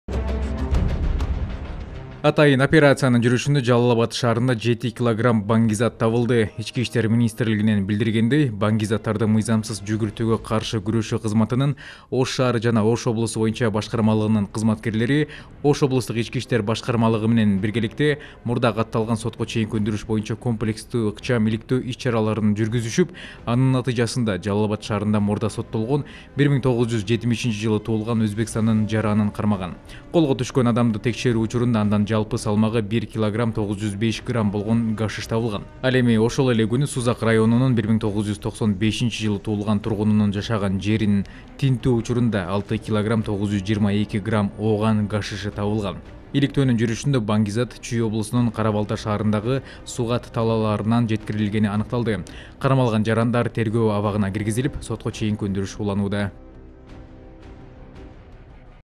Жалал-Абад шаарында эки адам 7 килограмм баңгизат менен кармалды
Атайын операциянын жүрүшүндө Жалал-Абад шаарында 7 килограмм баңгизат табылды.

Ички иштер министрлигинен билдиргендей баңгизаттарды мыйсамсыз жүгүртүүгө каршы күрөшүү кызматынын Ош шаары жана Ош облусу боюнча башкармалыгынын кызматкерлери Ош облустук ички иштер башкармалыгы менен биргеликте мурда катталган сотко чейинки өндүрүш боюнча комплекстүү ыкчам-иликтөө иш-чараларын жүргүзүшүп, анын натыйжасында Жалал-Абад шаарында мурда соттолгон 1970-жылы туулган Өзбекстандын жаранын кармаган. Колго түшкөн адамды текшерүү учурунда андан жалпы салмагы 1 кг 905 грамм болгон гашиш табылган.

Ал эми ошол эле күнү Сузак районунун 1995-жылы туулган тургунунун жашаган жерин тинтүү учурунда 6 кг 922 грамм ооган гашиш табылган. Иликтөөнүн жүрүшүндө баңгизат Чүй облусунун Кара-Балта шаарындагы сугат талааларынан жеткирилгени аныкталды. Кармалган жарандар тергөө абагына киргизилип, сотко чейинки өндүрүш уланууда.